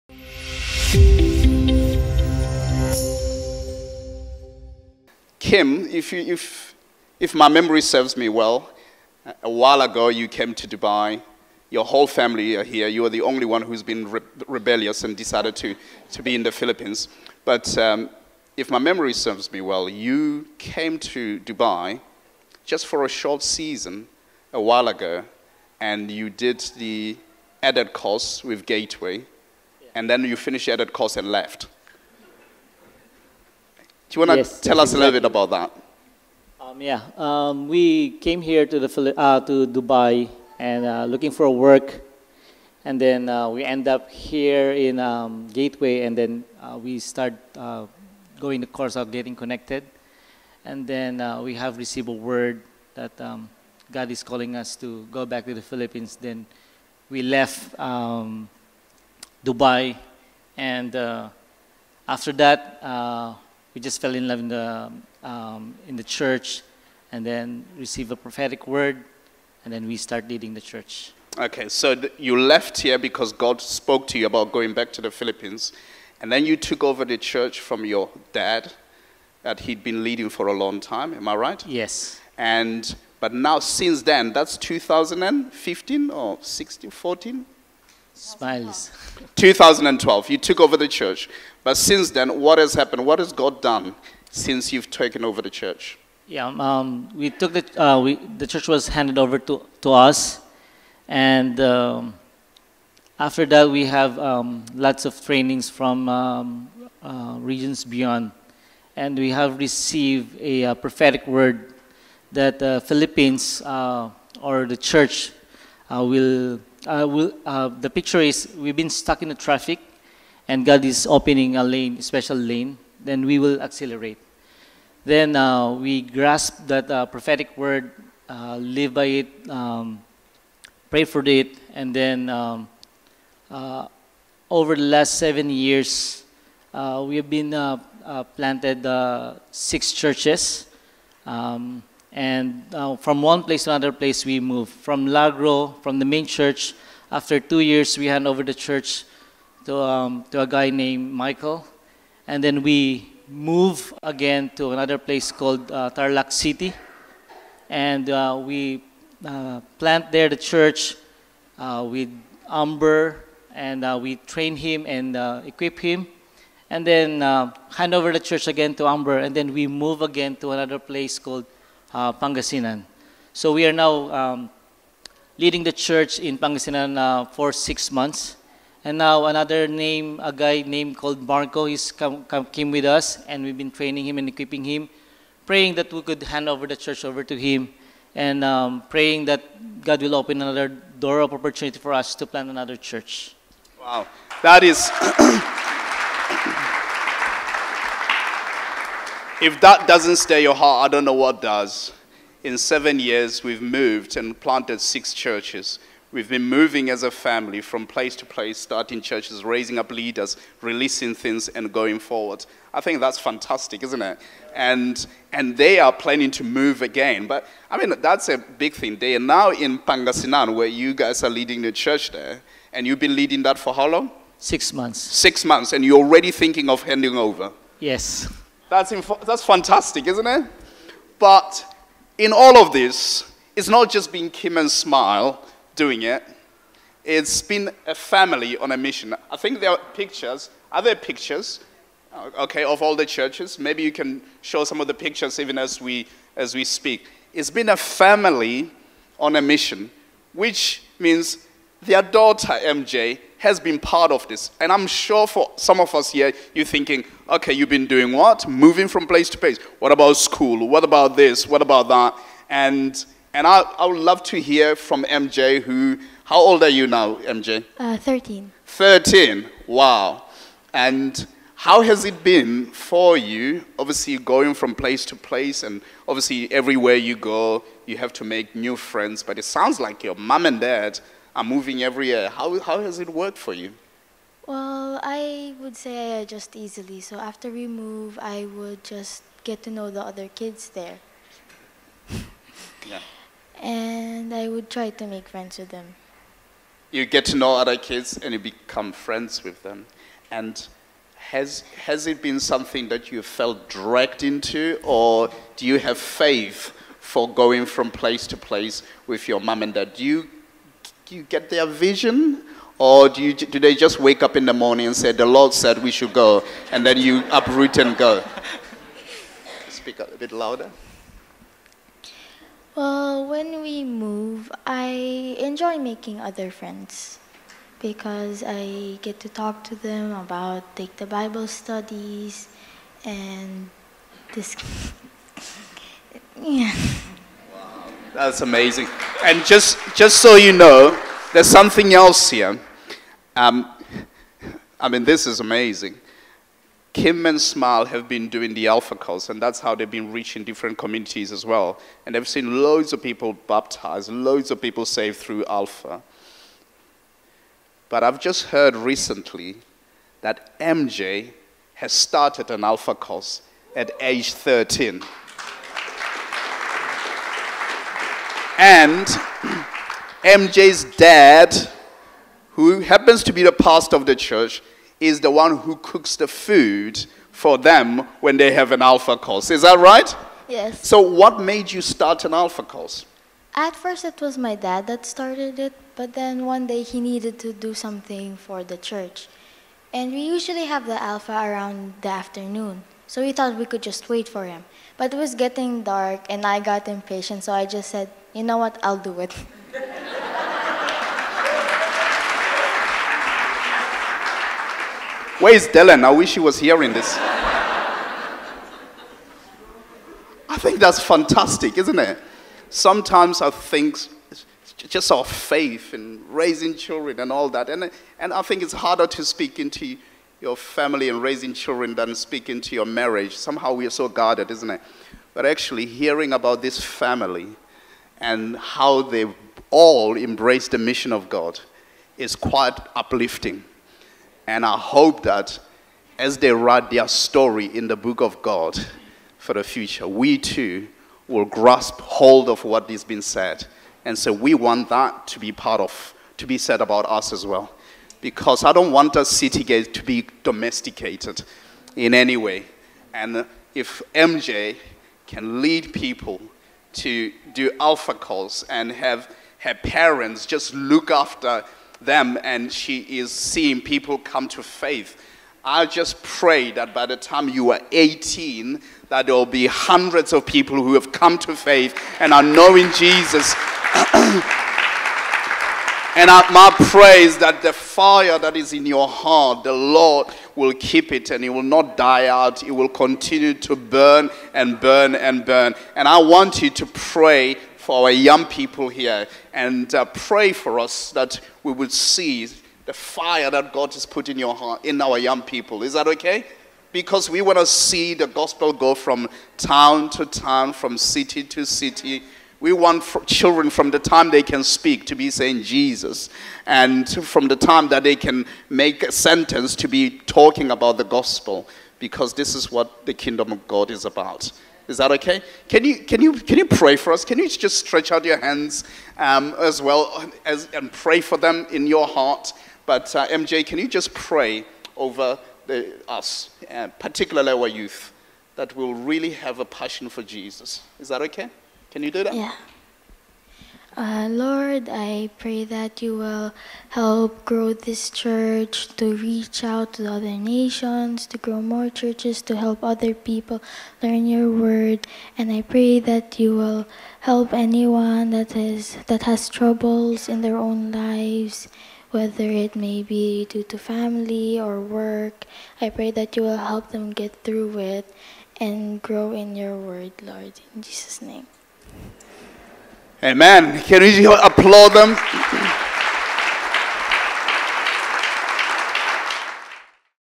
Mission, Family, and Gospel Advance in the Philippines | Regions Beyond Focus with the Jugos
This Sunday we interview Kim, Smile, and MJ Jugo who have been key catalysts for a movement of church planting across the Philippines. Over the course of their apostolic ministry, they have shepherded men and women, raising them up in their leadership and evangelical giftings. Hear more about what's happening in the Philippines and what God has in store for the nation.